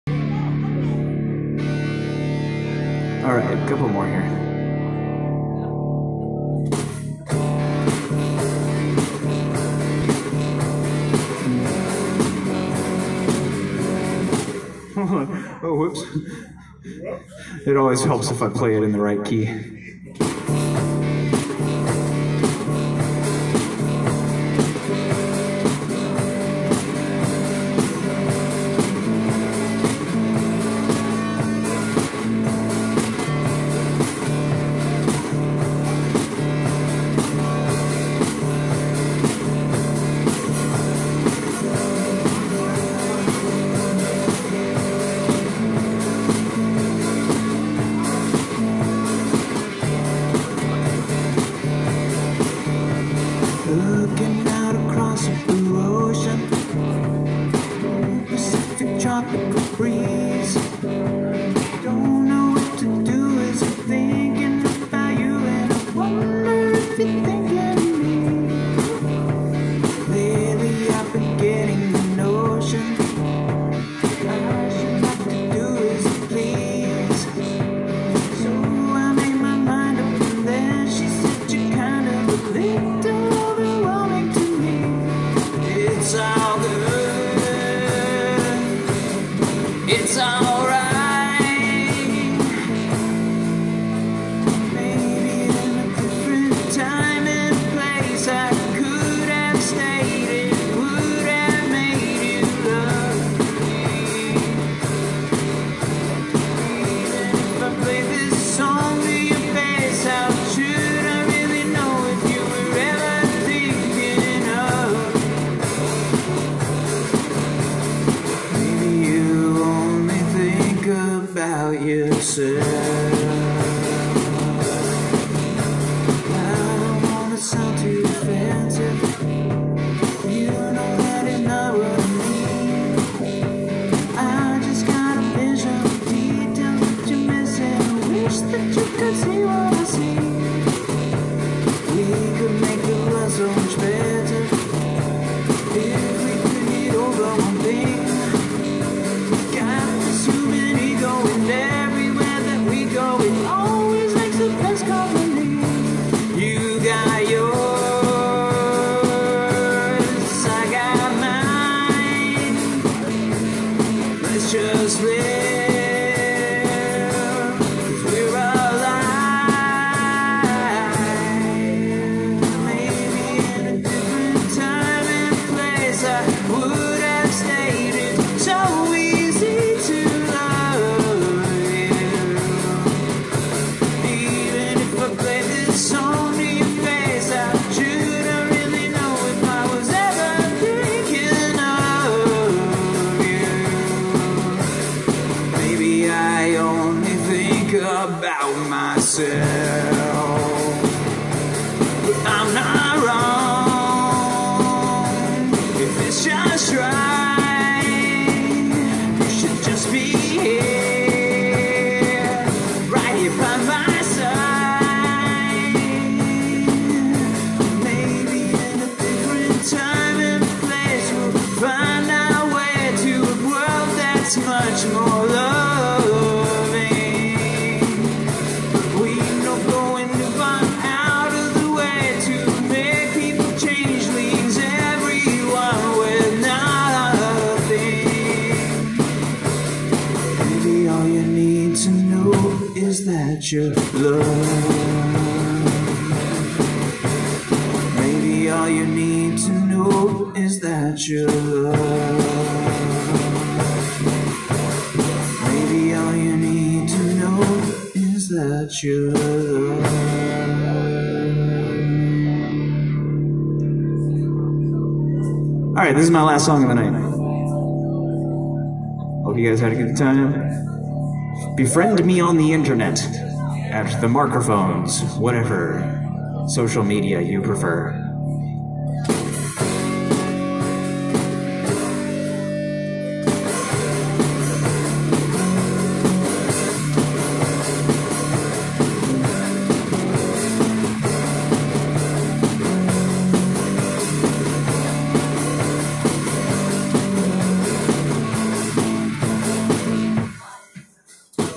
The Markrophones: Live Bremerton Washington 10/22/22 @ Deep Draft Brewing
The Markrophones one-man band performance, October 22, 2022, Deep Draft Brewing, Bremerton, WA, USA.